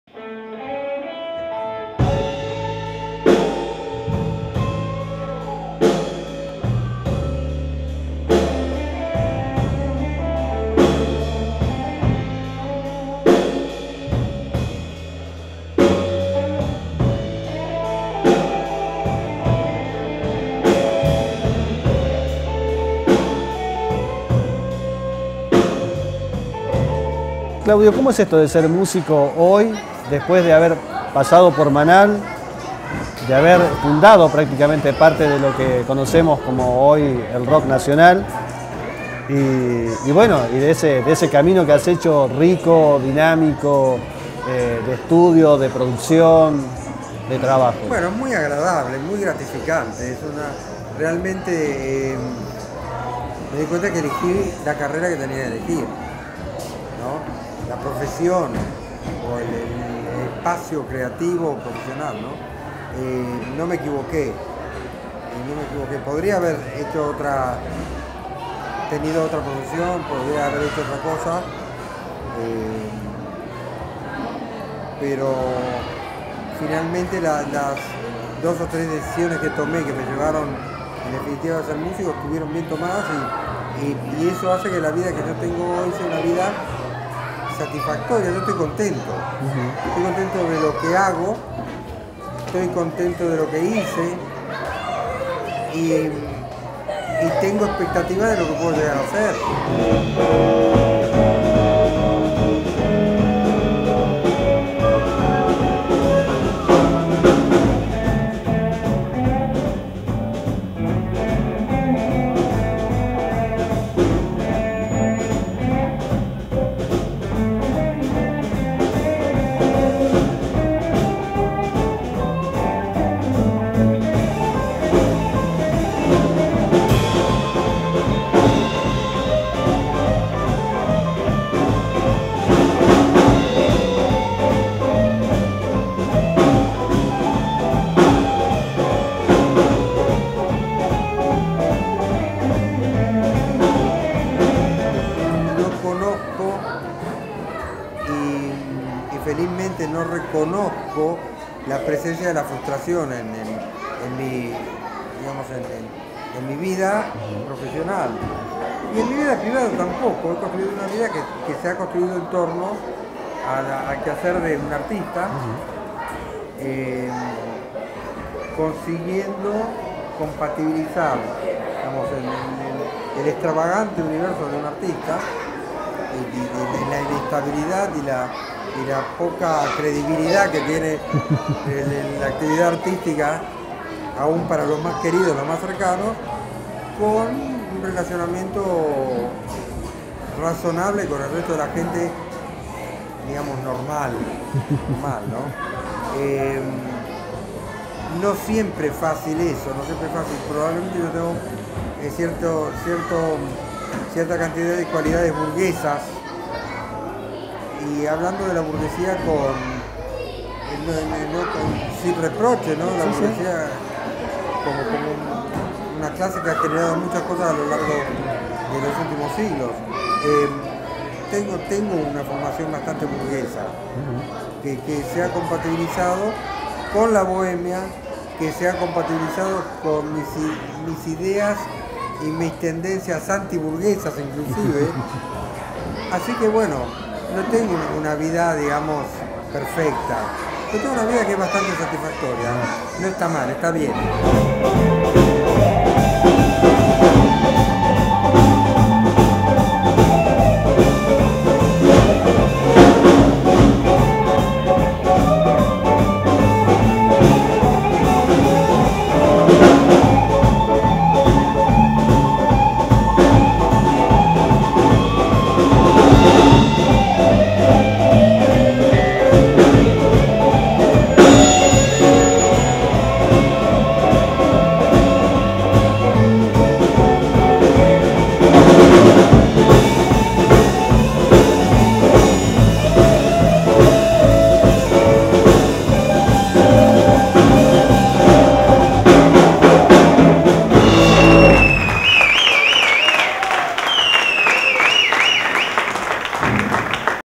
Claudio Gabis en concierto
Una conversación con Claudio Gabis, guitarrista de la primera formación de Los Abuelos de la Nada e integrante de Manal junto a Javier Martínez y Alejandro Medina, previo a su presentación en Concepción del Uruguay.